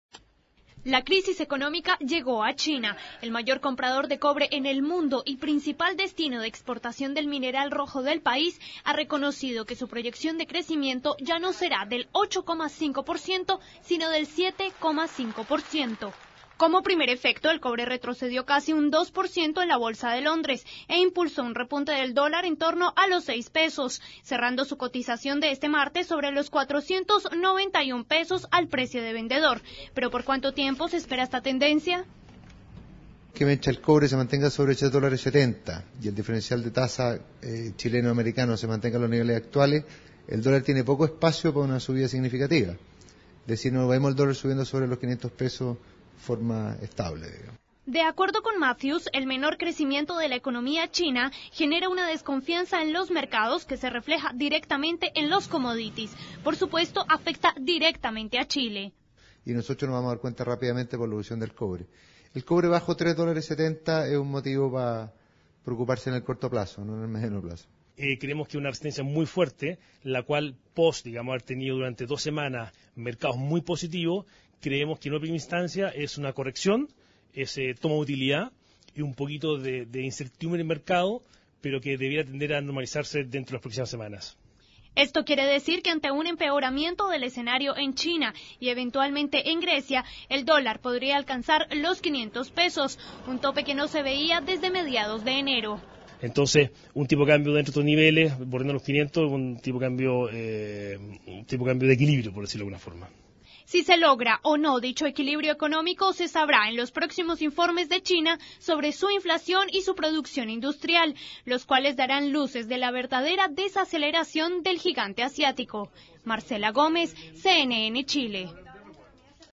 Crisis económica en China afectaría mercados mundiales
El mayor comprador del cobre, ha reconocido que su proyección de crecimiento bajó del 8, 5 por ciento a un 7, 5.